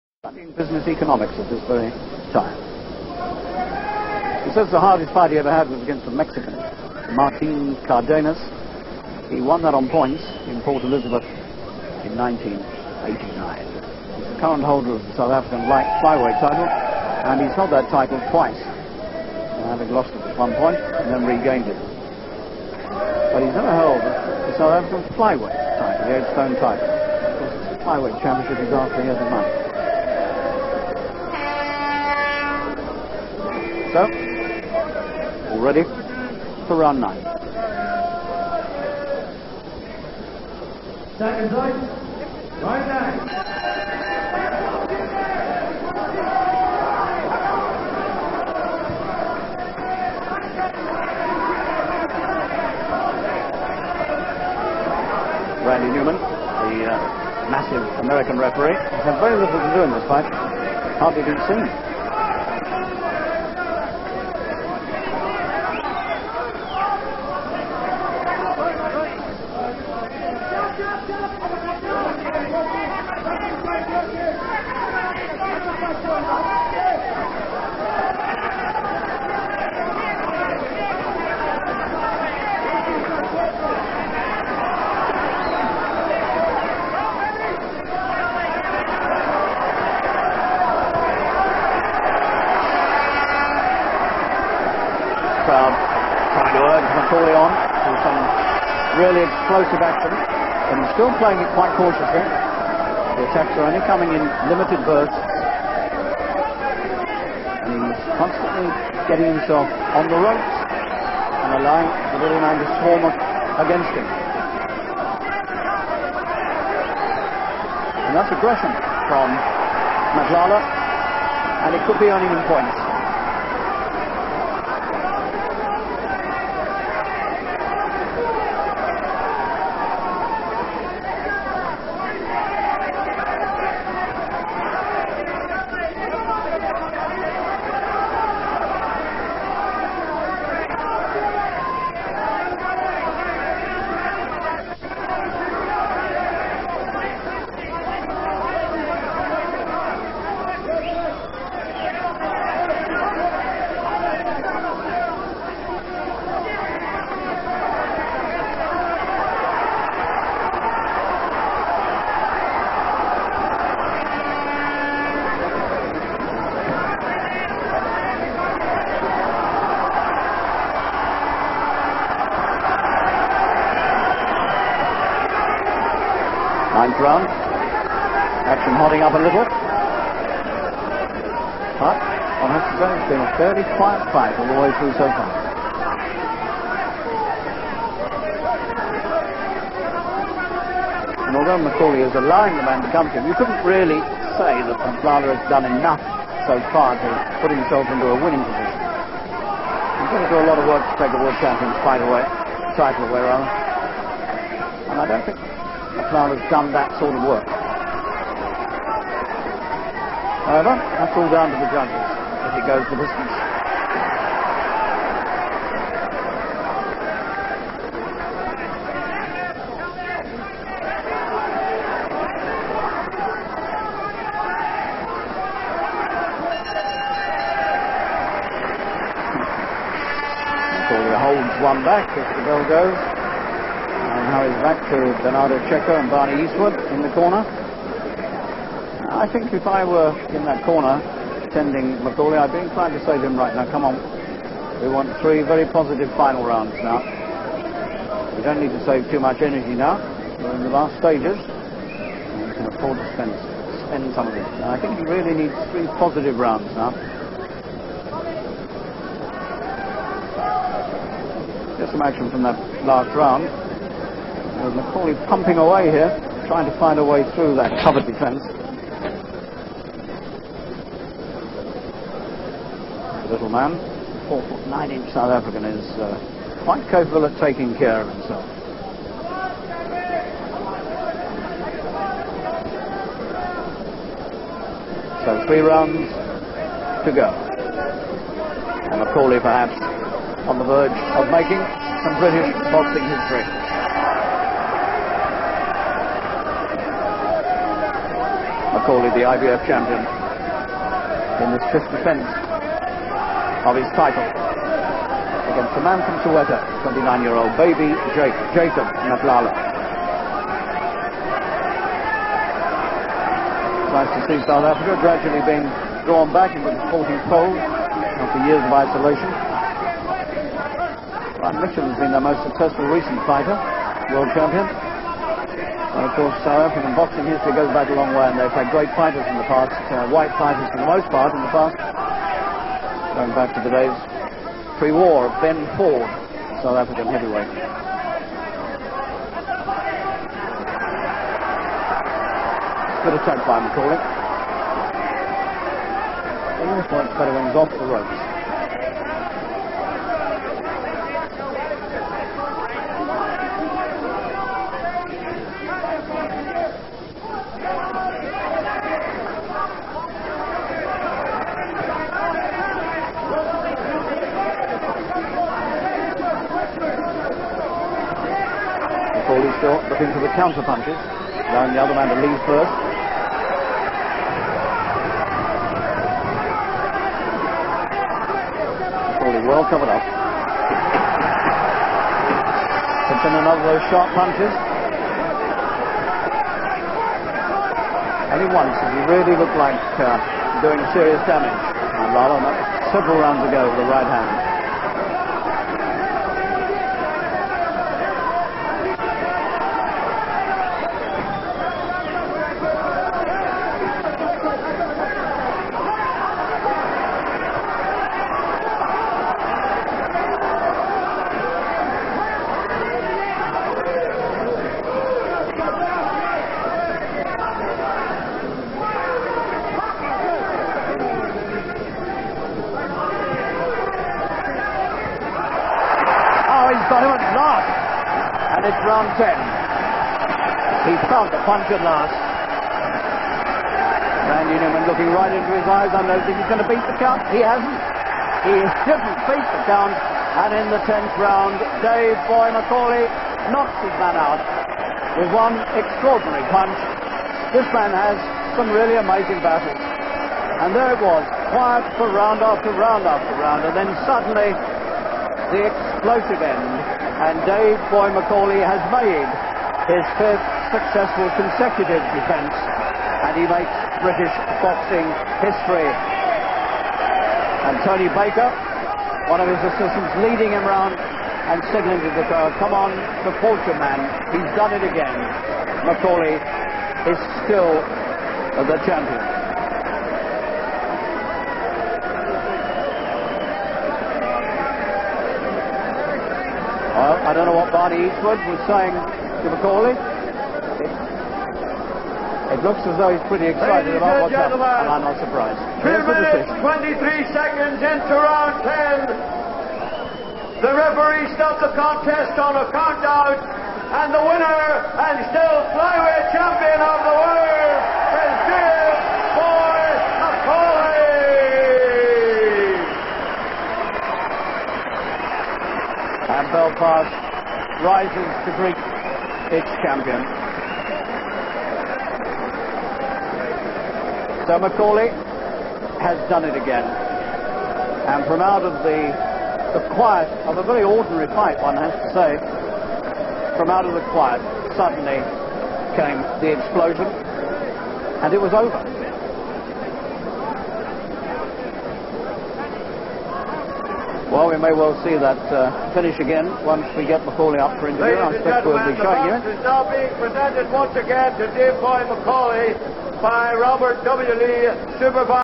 Dave McAuley vs Baby Jake Matlala rounds 9-10.flv
IBF flyweight title, Belfast 1991